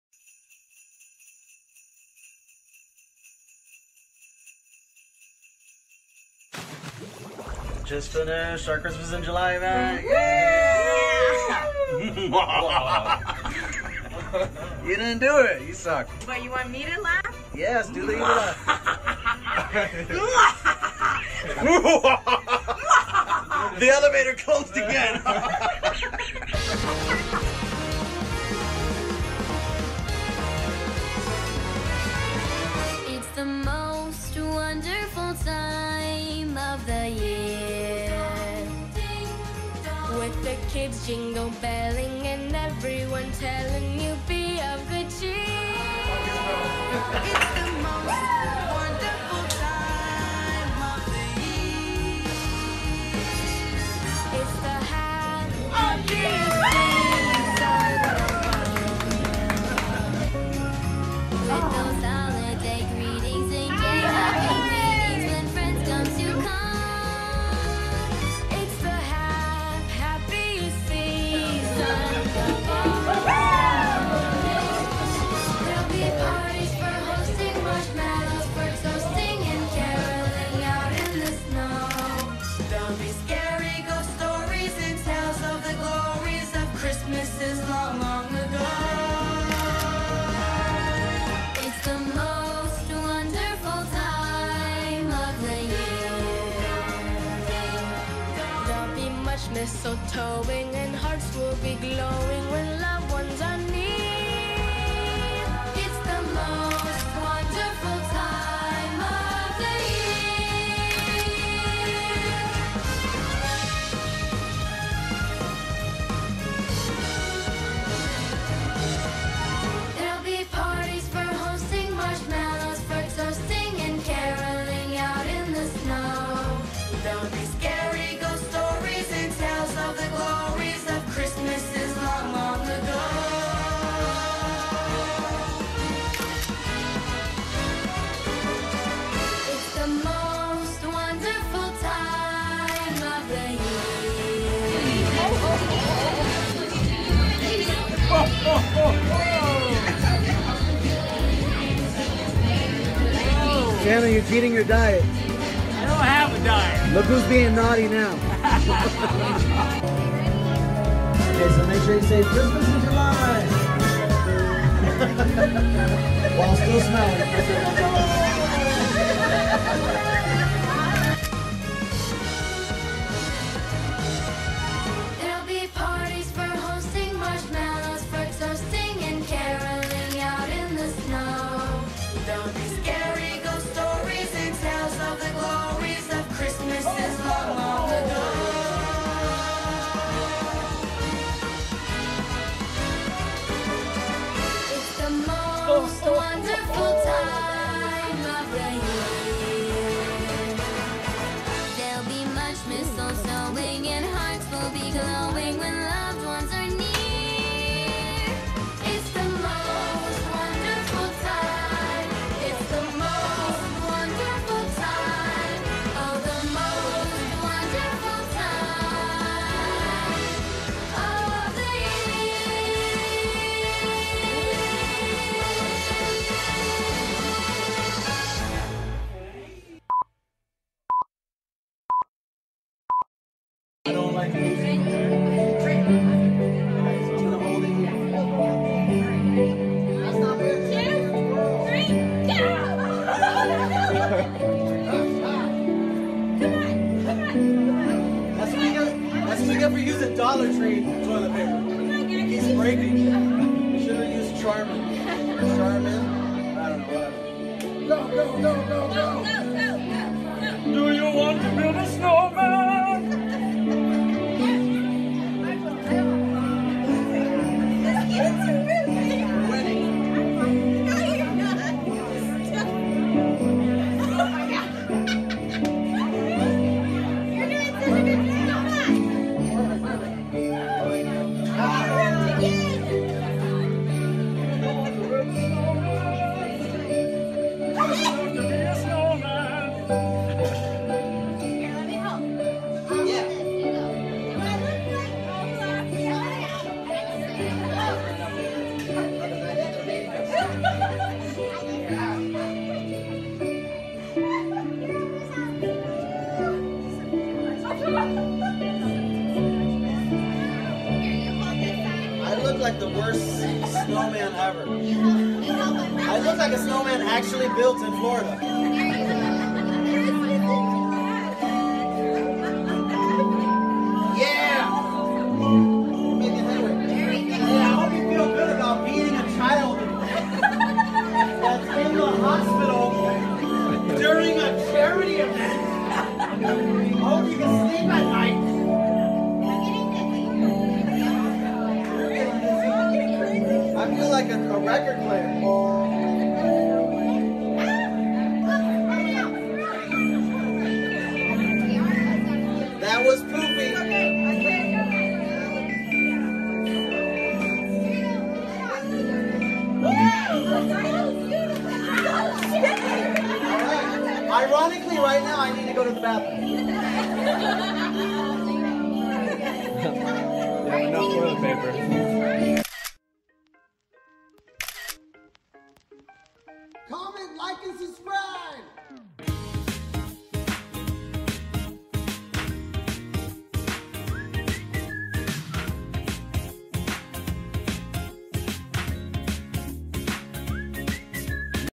7th Annual Christmas in July - Advent health - Helping Children 2019
With ALSO Charity Videos as our Charity Events take place.

Check out how our charity uses comedy and motivational videos to help children in need